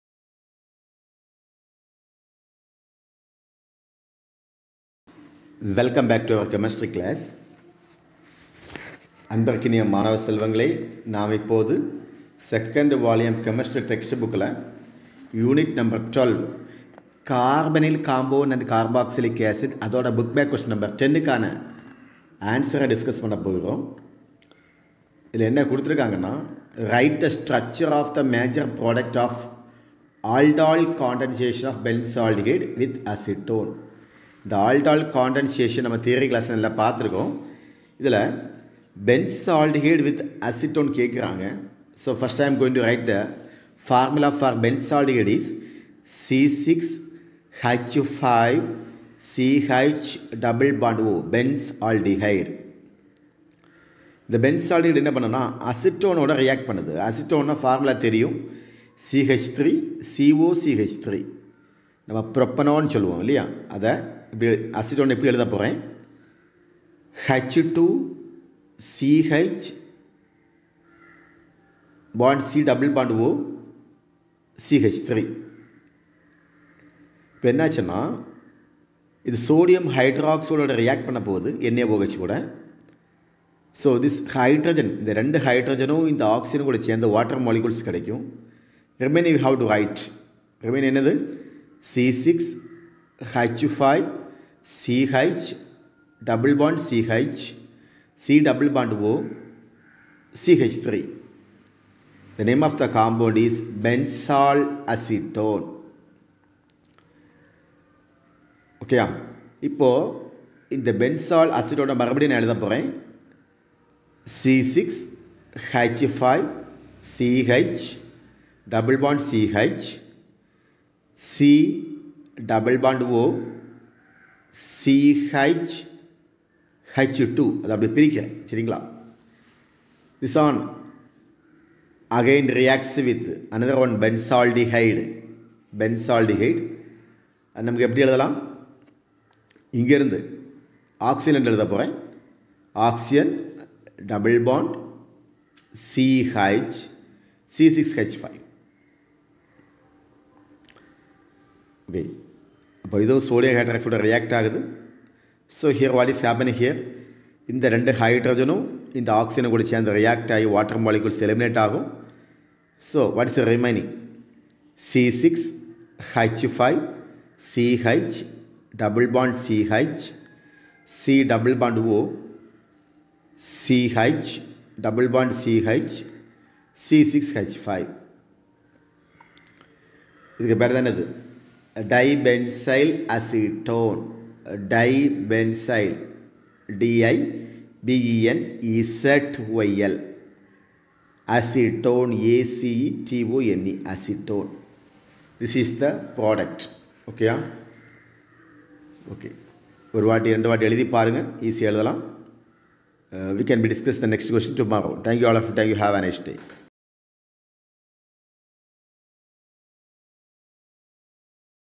Write the structure of major products of the aldol condensation of benzaldehyde with acetone /T & E
CHEMISTRY
            Grade 12
           volume 2
         unit 12
Carbonyl compounds and Carboxylic acids
   Book back question no 10
  I uploaded Chemistry Volume 2 Grade 12 Carbonyl compounds and Carboxylic acids book back question no 10